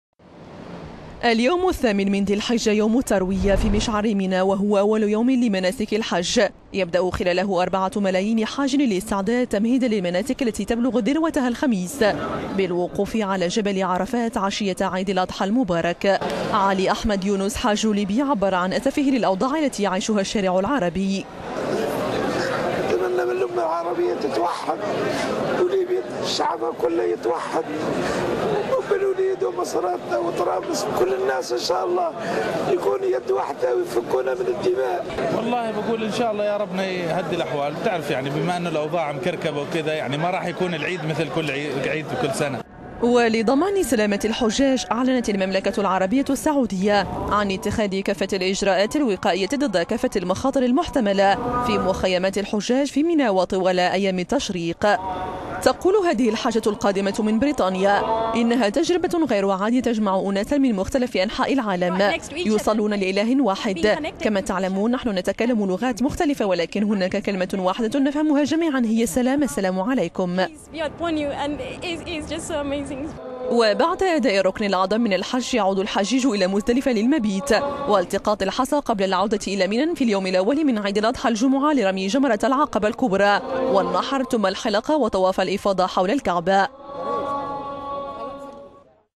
مليونا حاج في منى بيوم التروية
اليوم الثامن من شهر ذي الحجة الهجري، يوم التروية في مشعر منى، وهو أول يوم لمناسك الحج يبدأ خلاله أربعة ملايين حاج الاستعداد تمهيدًا للمناسك التي تبلغ ذروتها الخميس بالوقوف على جبل عرفات عشية عيد الأضحى المبارك.

هذه السنة وبسبب الأوضاع الأمنية التي تعيشها كل من سوريا وليبيا عقب الربيع العربي لم يتمكن المواطنون الراغبون في تأدية مناسك الحج من ذلك بسبب أوضاعهم الصعبة جراء الظروف التي تمر بها المنطقة حالياً.

علي أحمد يونس حاج ليبي عبر عن أسفه للأوضاع التي يعيشها الشارع العربي ودعا الى توحيد الأمة العربية والاسلامية.

أبو سناء مواطن سوري مقيم في المملكة السعودية كان محظوظا بأداء الركن الخامس من أركان الاسلام.

ولضمان سلامة الحجاج أعلنت المملكة العربية السعودية عن اتخاذ كافة الإجراءات الوقائية ضد كافة المخاطر المحتملة في مخيمات الحجاج في منى وطوال أيام التشريق.

حاجة قادمة من بريطانيا تقول :"انها تجربة غير عادية تجمع أناسا من مختلف أنحاء العالم جنبا الى جنب ويصلون لاله واحد مرتبطون مع بعضهم البعض. لأنه، كما تعلمون، نحن نتكلم لغات مختلفة ولكن هناك كلمة واحدة نفهمها جميعا هي "السلام" السلام عليكم. "

وبعد أداء الركن الأعظم من الحج يعود الحجيج إلى مزدلفة للمبيت والتقاط الحصى قبل العودة إلى منى في اليوم الأول من عيد الأضحى الجمعة لأداء  بقية المناسك.